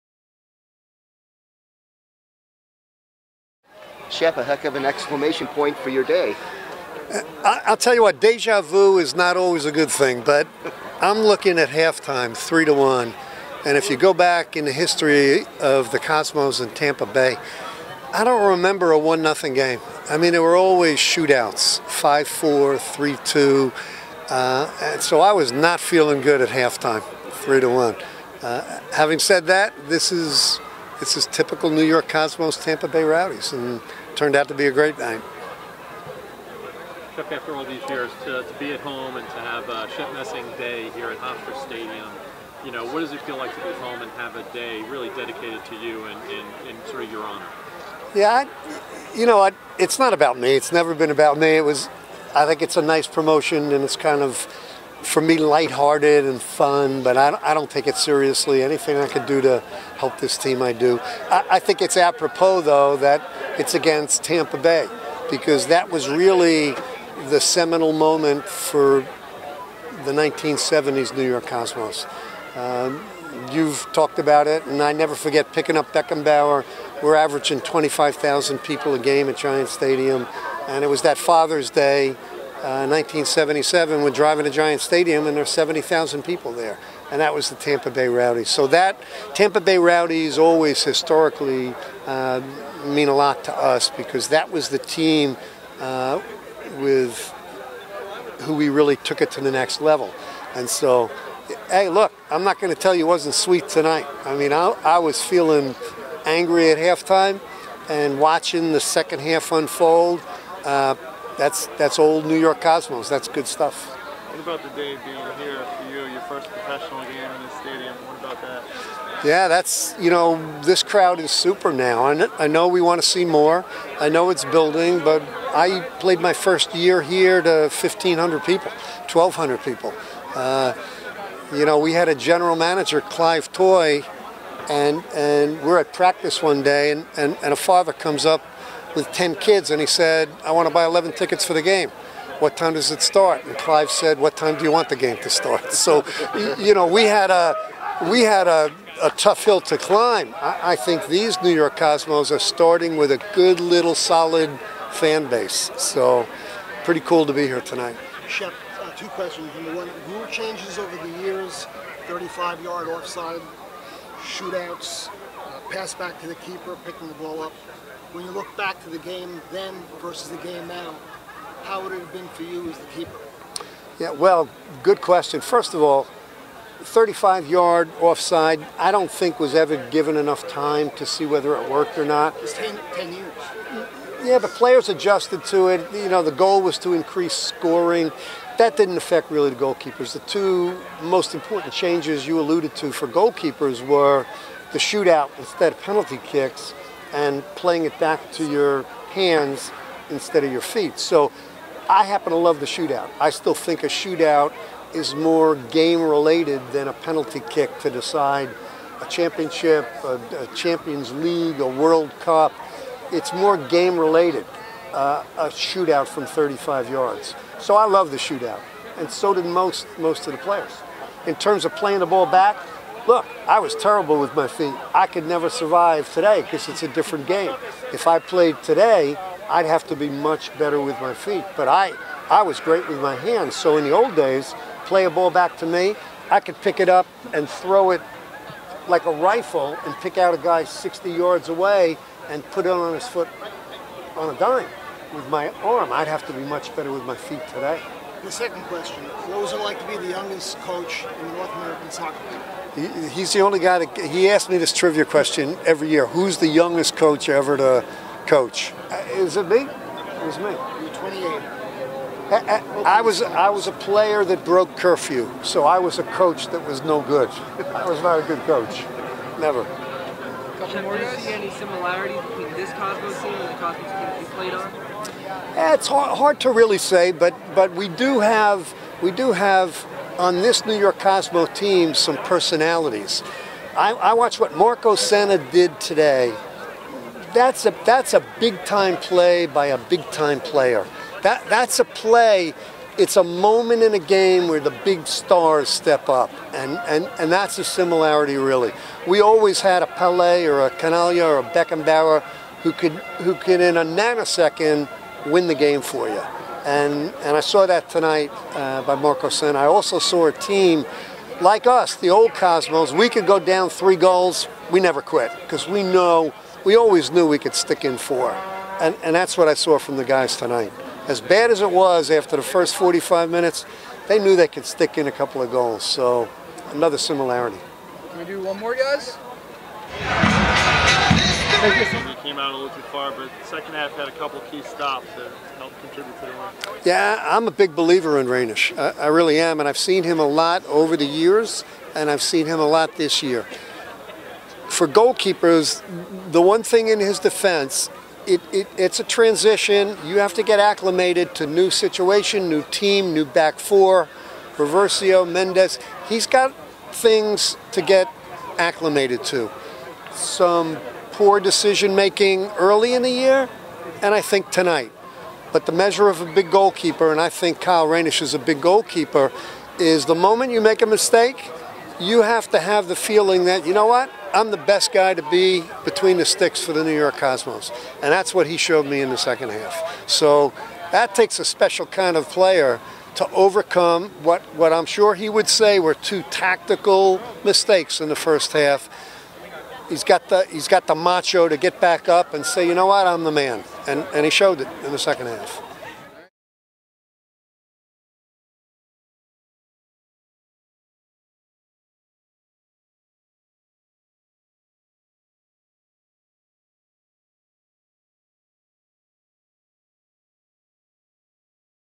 Shep Messing ~ New York Cosmos 9/29
Shep Messing chats with the New York Media after Shep Messing day with the New York Cosmos on 9/29